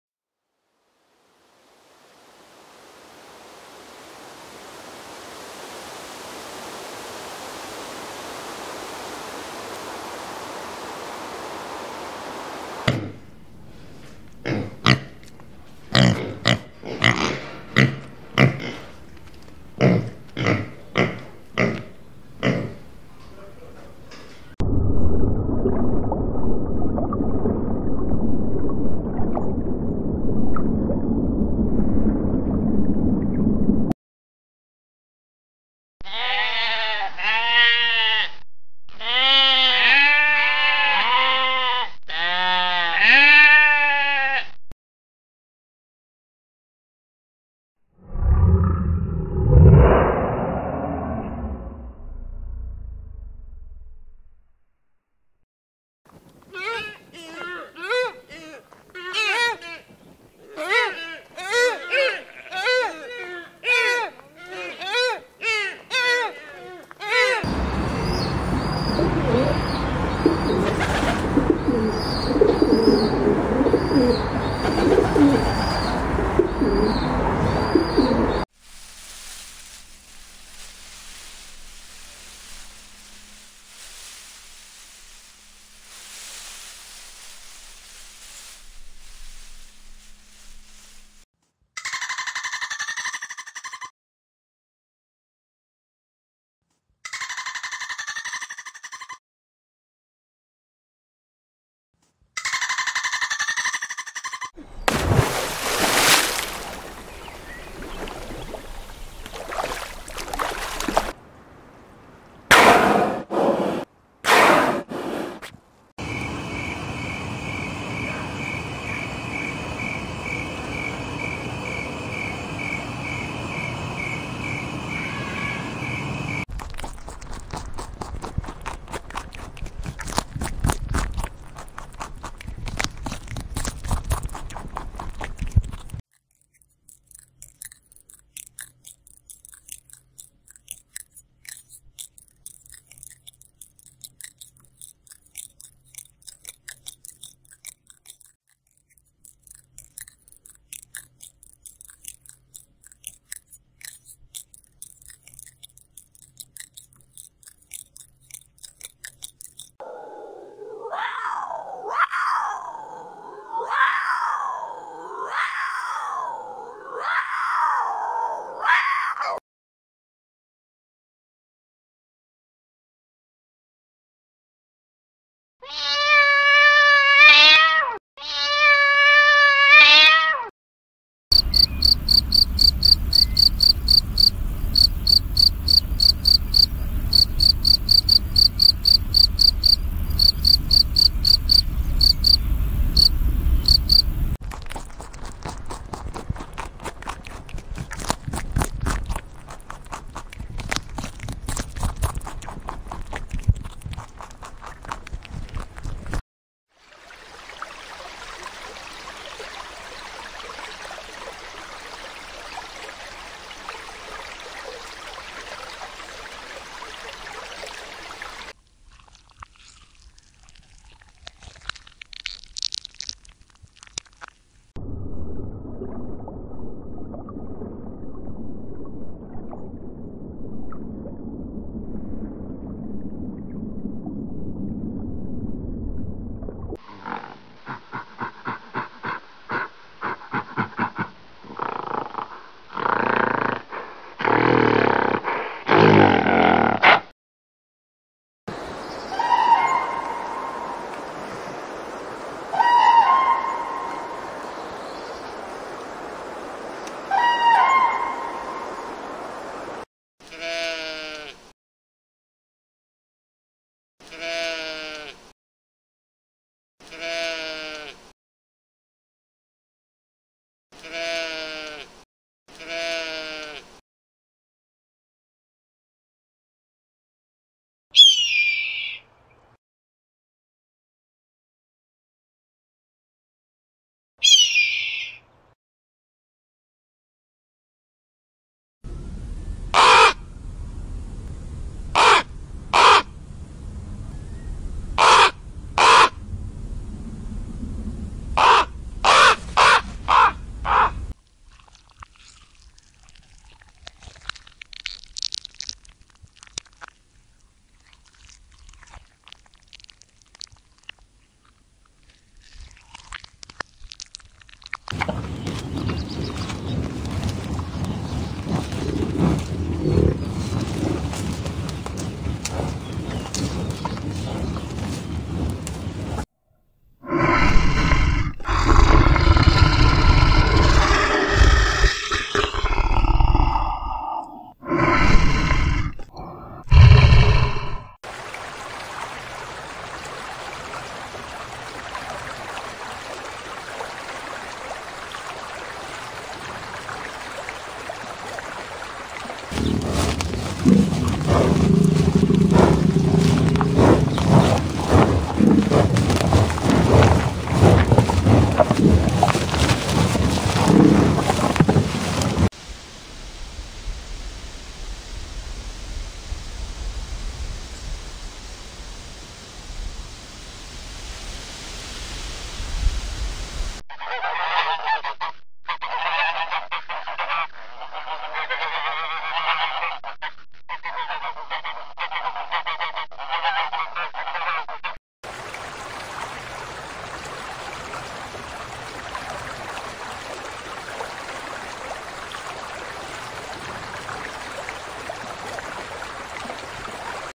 nama nama hewan bahasa indonesia dan bahasa inggris || part 3
video ini belajar untuk lebih mengenal nama nama hewan dengan bahasa indonesia dan bahasa inggris.tonton video selalu di channel kozoo animal ya